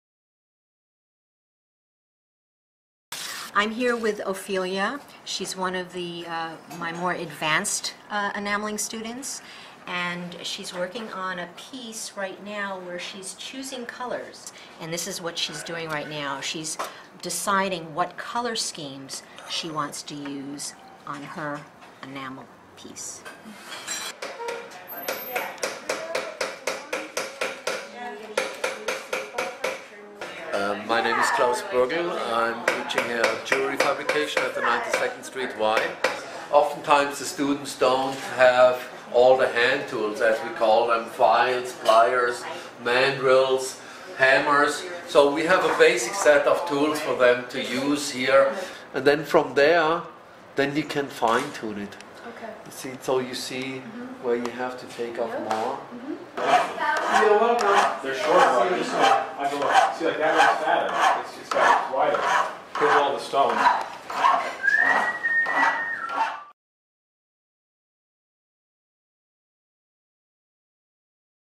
Jewelry Studios at the 92nd Street Y
The 92nd Street Y's jewelry studios are the most modern and fully equipped in New York City. Students are required to purchase their own basic tools. Suggested tools, supply lists or a department calendar are available online or upon registration.